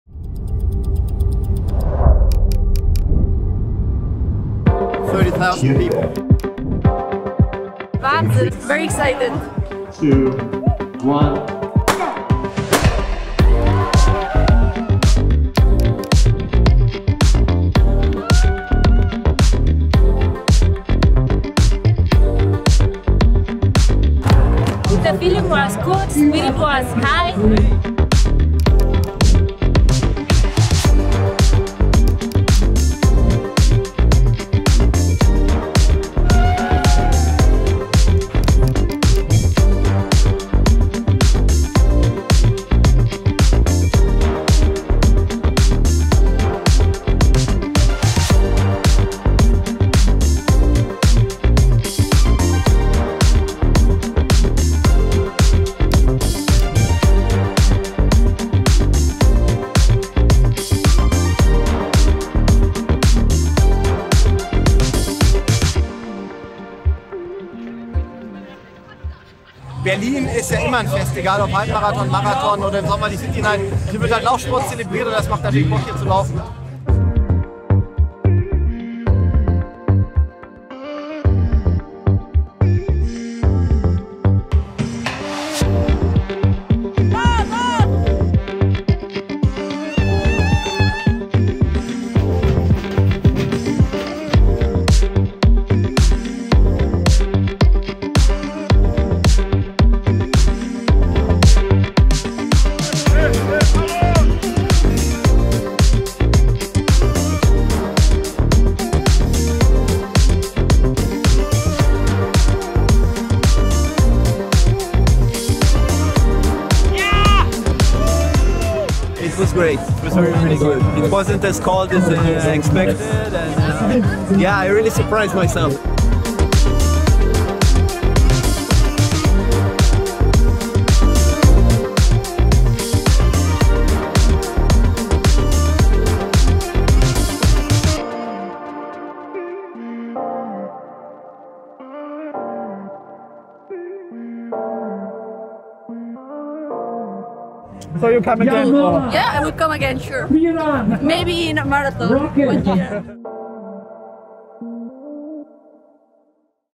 Best of Generali Berliner Halbmarathon 2022
Sonne, Wind, Schnee - Über 30.000 Läuferinnen und Läufer trotzten Wind und Wetter und bezwangen die 21.097km des größten deutschen Halbmarathons.
Bei der 41. Auflage des Generali Berliner Halbmarathons gingen Alex Kibet und Sheila Chepkirui Kiprotich als Erste über die Ziellinie. Der Kenianerin gelang es ebenfalls den alten Streckenrekord von 2019 auf eine Zeit von 1:05:02 zu verbessern.
Herzlichen Glückwunsch an alle Sieger und Finisher und vielen Dank an die tausenden Zuschauer, die wieder einmal für eine atemberaubende Stimmung sorgten!